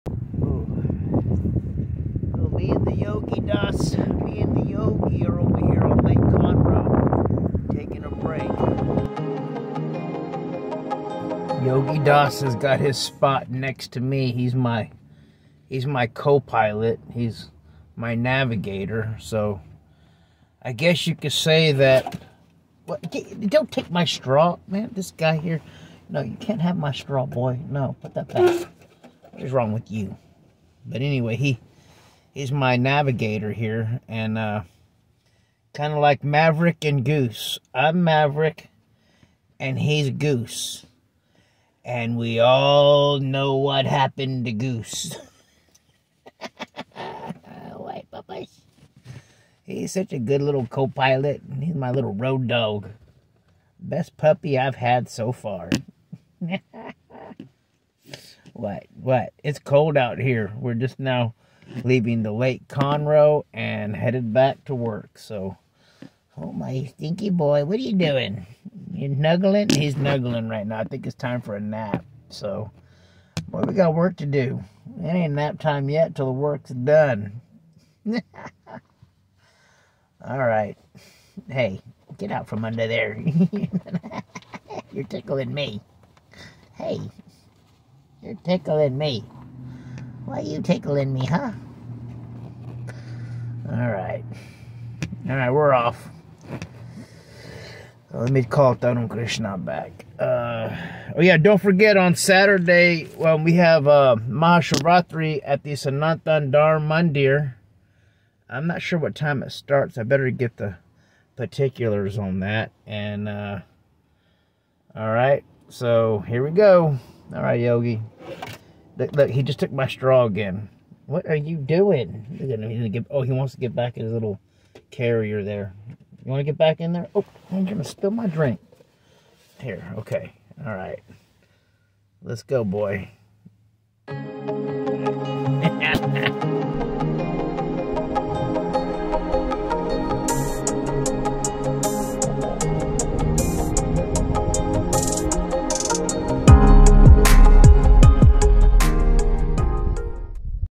Yogi Das's first time at Lake Conroe
We took a lunch break today at lake Conroe today and this little furball had the time of his little life.
It was freezing cold! He absolutely loved it.
Rabbits thrive in cold weather!